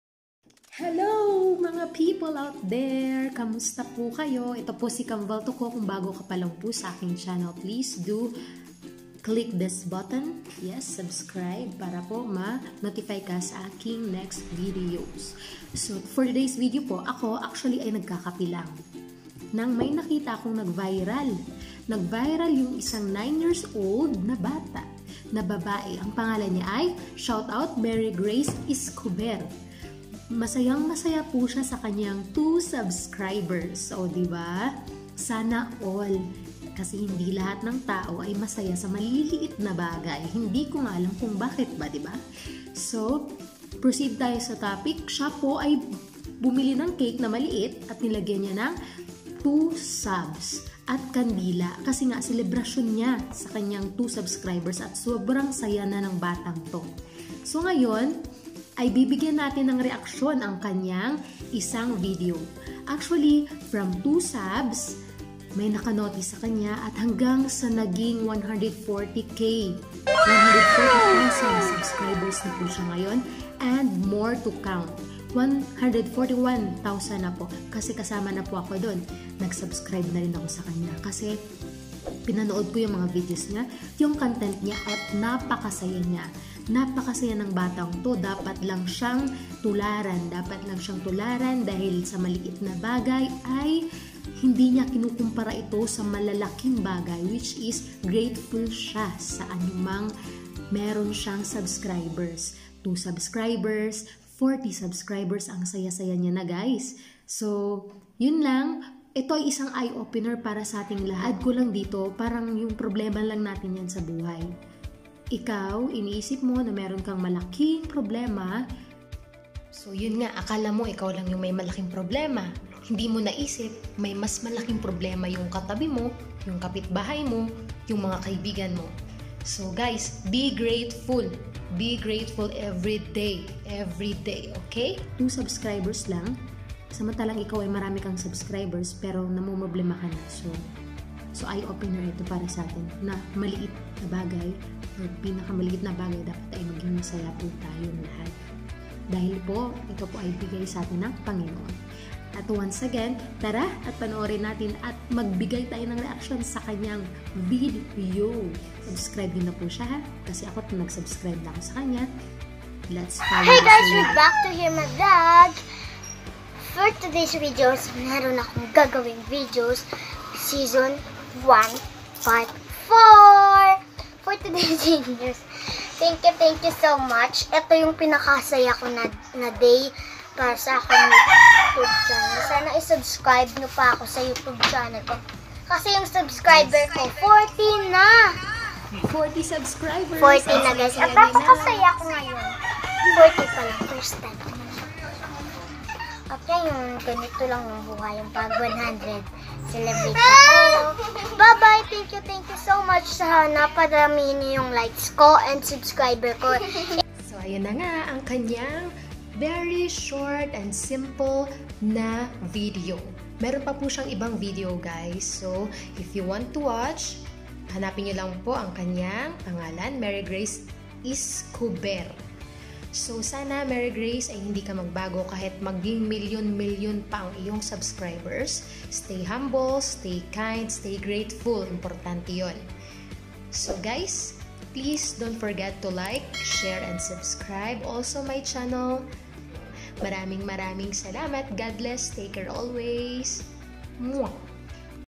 My reaction to MARY GRACE ESCOBER||TRENDING CONGRATULATIONS "2 SUBCRIBERS JUMP TO 140K PLUS SUBS"😊
Ang batang ito ay subrang grateful sa kanyang maliit palang na achievement dahil nagkaroon sya ng 2 subscribers sa youtube sya ay subrang natuwa at napabili ng cake at coke para e celebrate ang 2 subscribers, kaya naman marami ang natuwa sa kanya at nag subscribe at ngayon ay umavot na sa 140k plus ang kanyang tagapanuod.. Stay humble and grateful Mary grace Escober😊cheers.